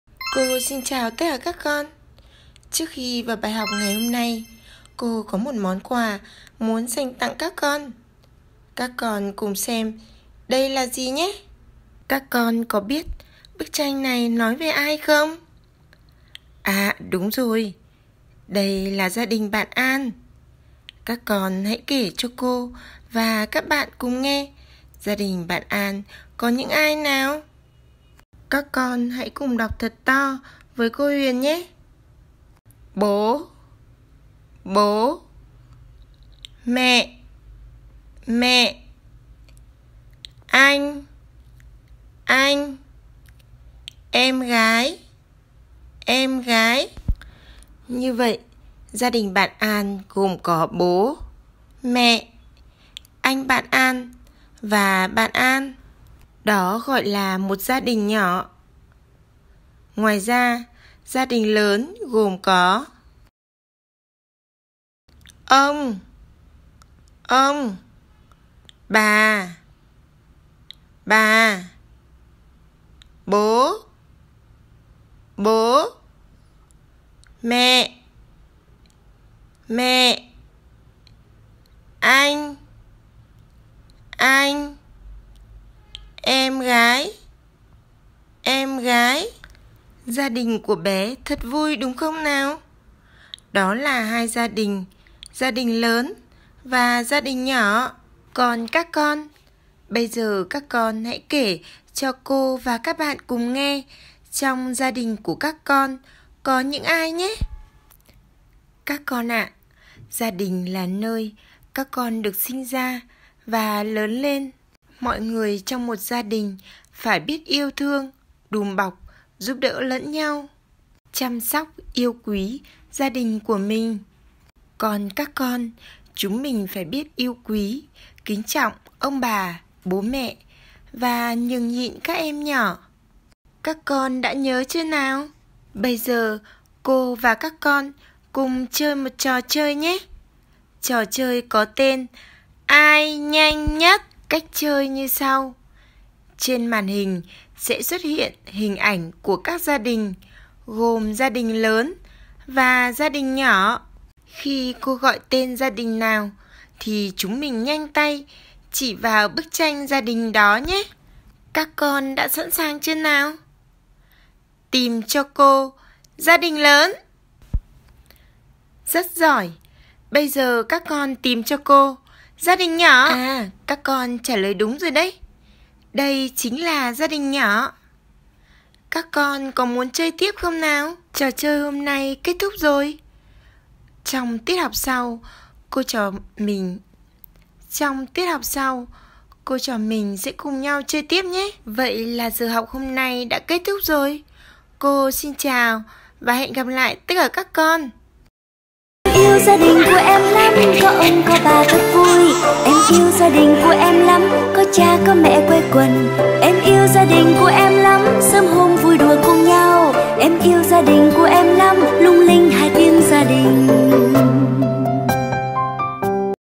Dạy bé tập nói các thành viên trong gia đình- HĐKP Gia đình thân yêu của bé
Hoạt động khám phá gia đình thân yêu - dạy bé tập nói các thành viên trong gia đình. Dạy bé tập nói về gia đình của bé